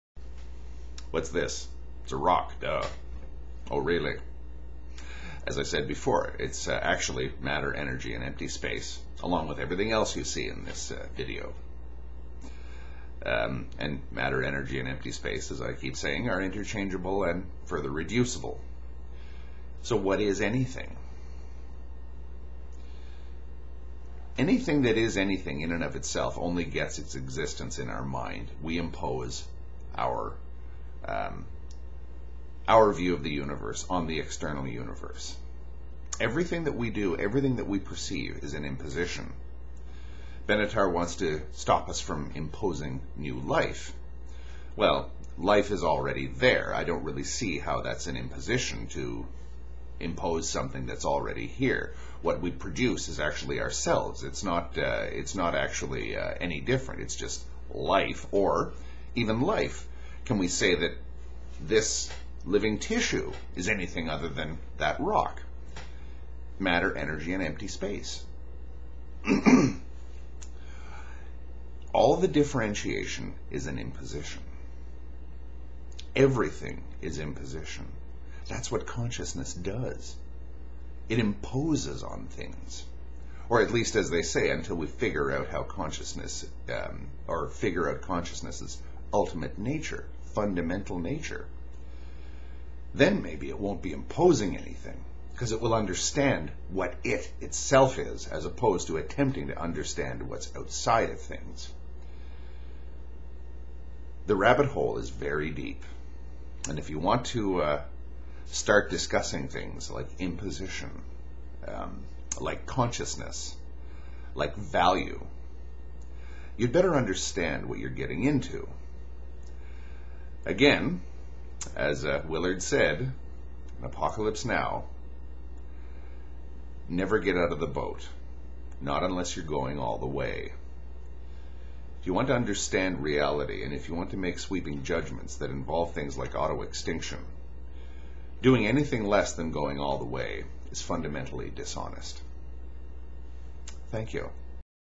Antinatalism and Depression LXXXV: Imposition
Benatar wants us to stop imposing. Let's try and stop imposing and see what happens.

"but you must not eat from the tree of the knowledge of good and evil, for when you eat of it you will surely die." - Genesis 2:17
What was so bad about the knowledge of good and evil?
Split the universe into separate "things" and it stops making sense. It stops making any sense whatsoever.

contrast that with:

"Platonic Ideas and Forms are noumena, and phenomena are things displaying themselves to the senses [...] that noumena and the noumenal world are objects of the highest knowledge, truths, and values is Plato's principal legacy to philosophy."
—The Oxford Companion to Philosophy (Thanks to Wiki)